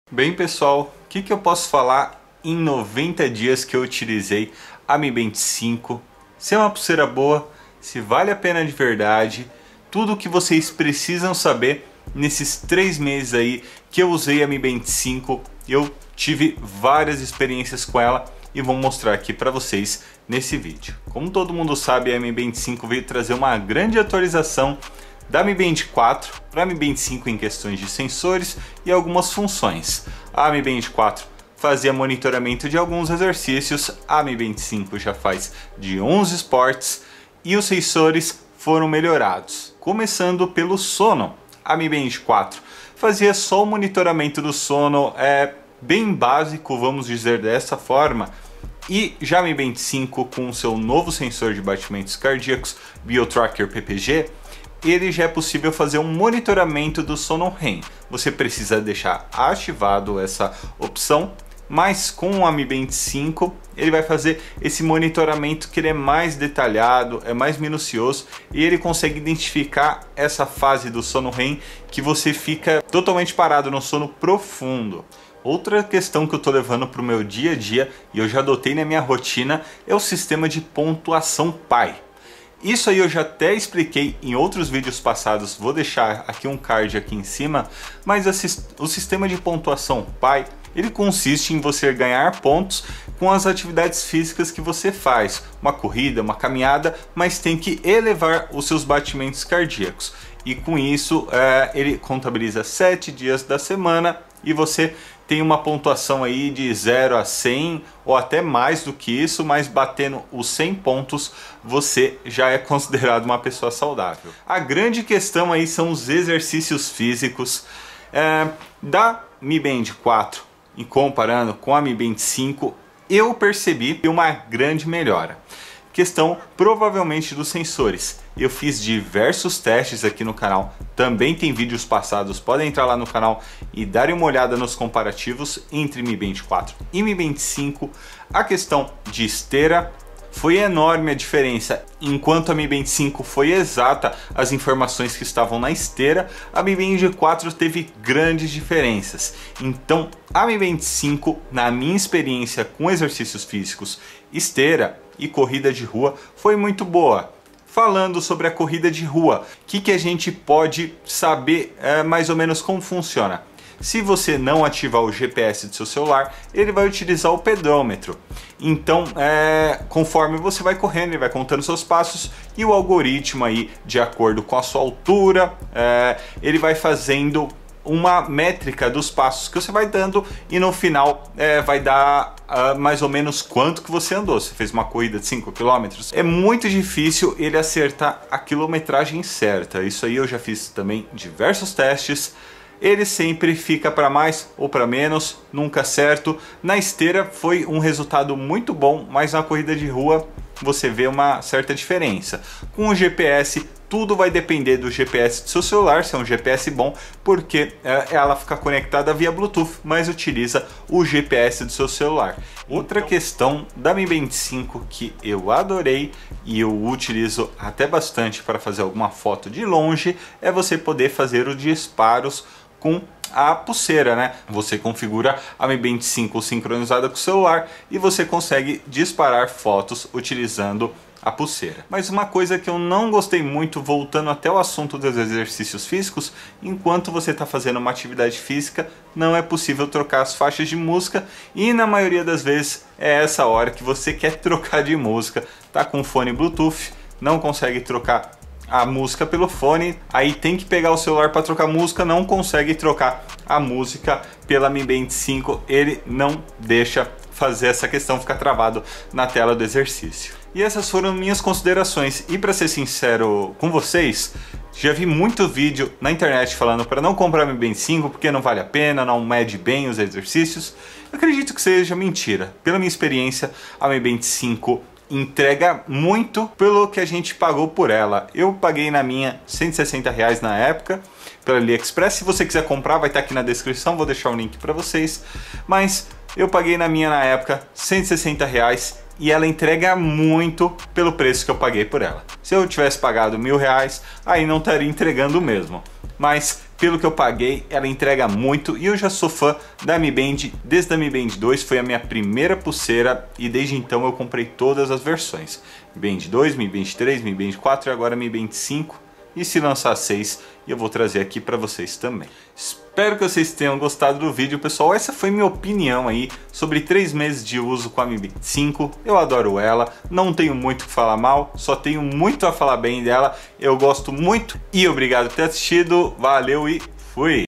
MI BAND 5 com 3 Meses de Uso | Minha Opinião
Depois de 3 meses usando a Mi Band 5 todos os dias queria dar minha opinião sincera sobre essa Smartband.

Compre a Mi Band 5